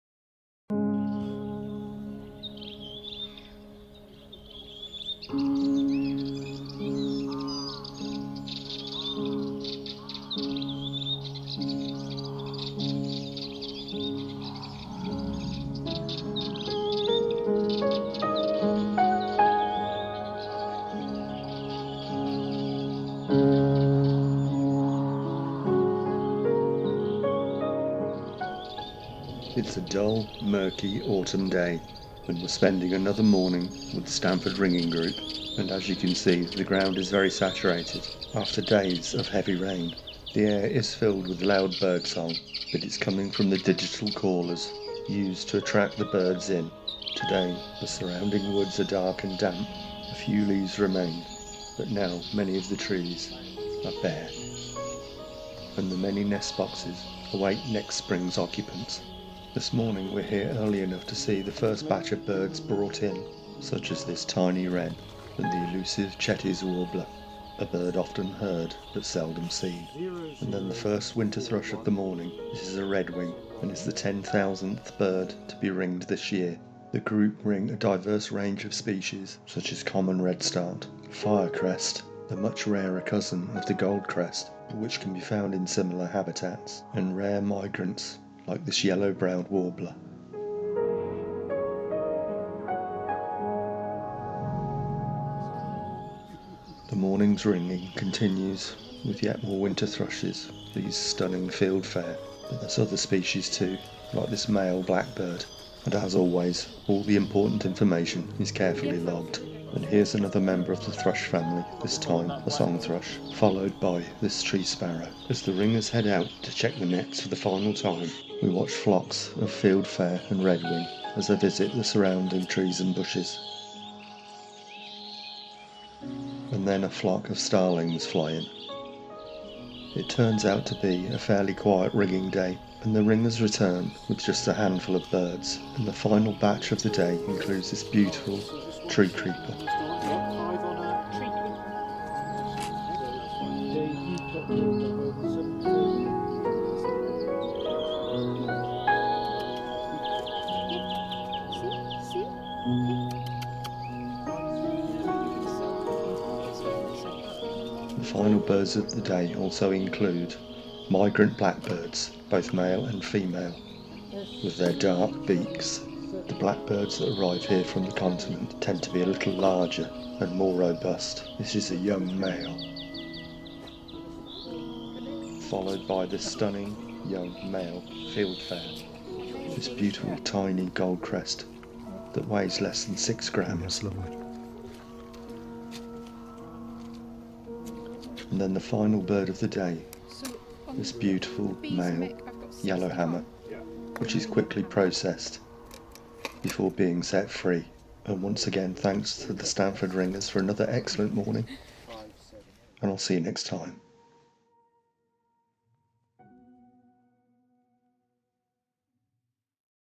Stanford Ringing Group: 24th November 2019
Film of a dark murky autumn morning spent with the Stanford Ringing Group on the day they ringed their ten thousandth bird of 2019, amazing.